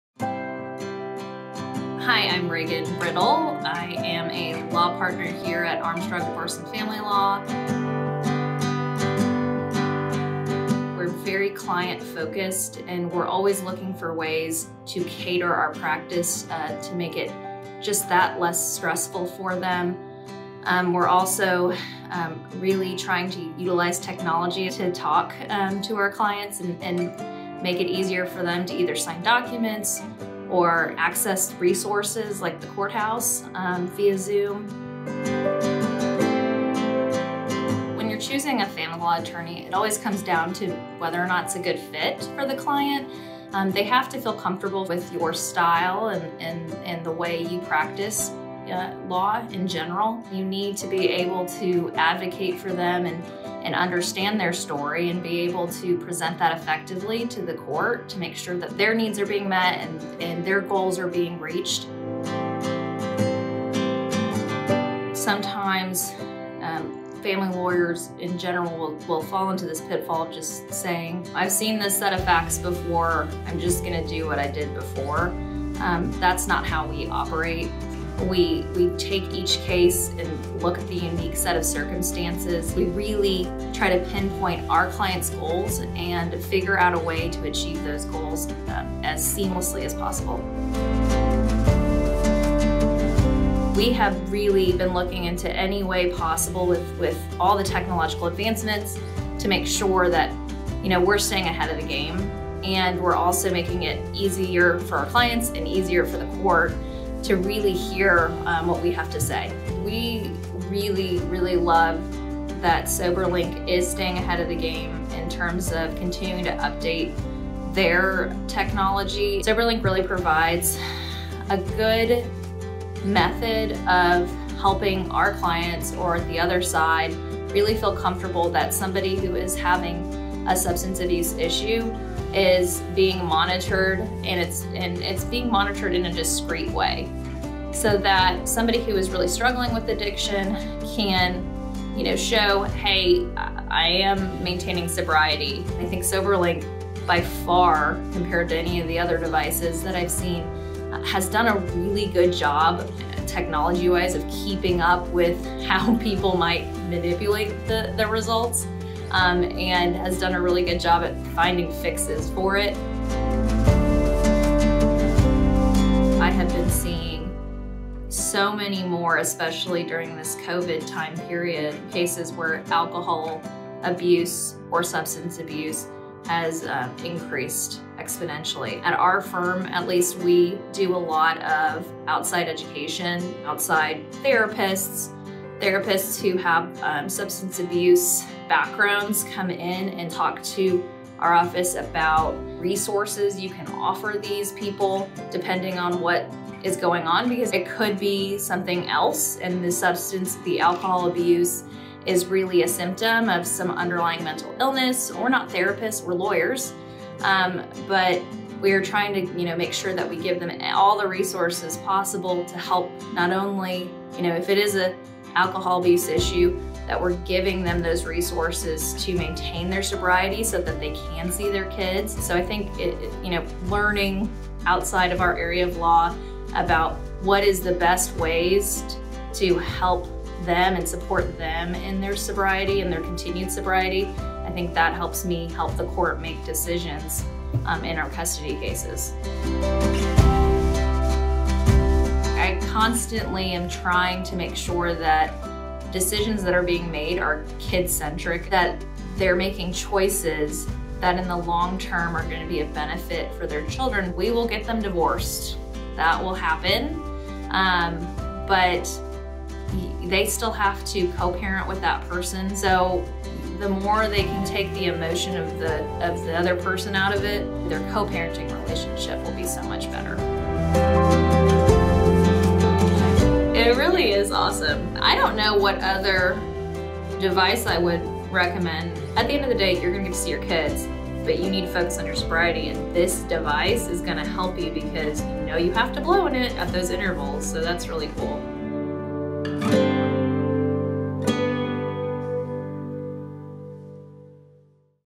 'Kid-Centric' Litigation: A Family Lawyer Discusses Her Firm's Unique Approach for Alcohol Misuse
Attorneys at Armstrong Divorce and Family Law are committed to being 'client-focused' and 'kid-centric', and Reagan Riddle, a partner at the firm, is no exception. With over 55,000 custody cases involving alcohol misuse each year, Riddle uses Soberlink as a method to help her clients either maintain parenting time or obtain peace of mind that their children are being supervised by a sober parent. She shares, "I don't know what other device I would recommend."